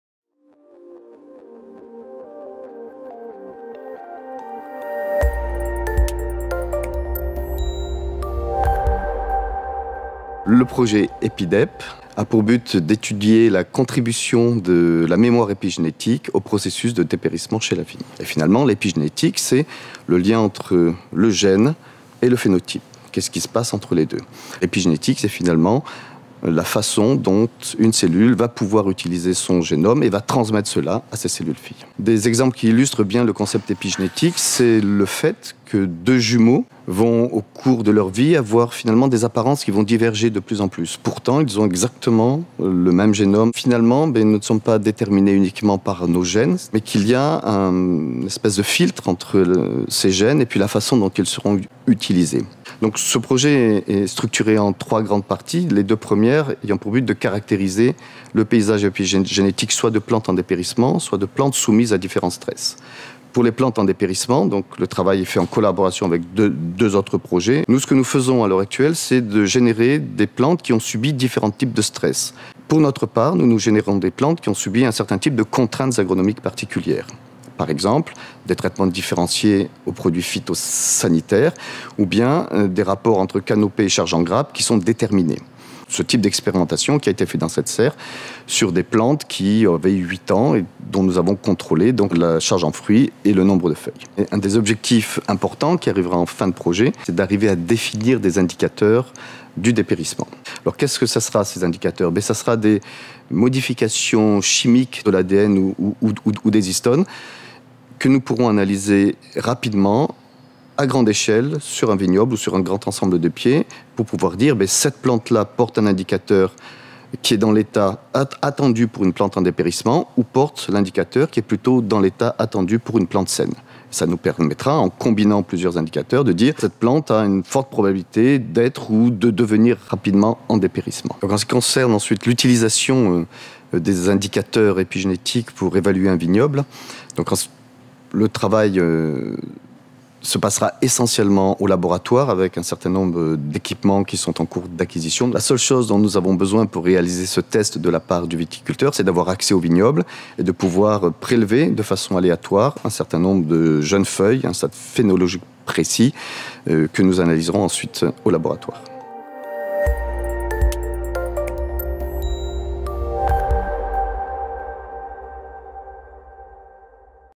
Epidep : contribution de la mémoire épigénétique au dépérissement de la vigne
EPIDEP étudie le lien entre l’évolution des épigénomes de la vigne et le processus de dépérissement. Le programme de recherche combine l’analyse du statut épigénétique de la plante à l’étude de sa physiologie en relation avec les pratiques culturales et les contraintes environnementales.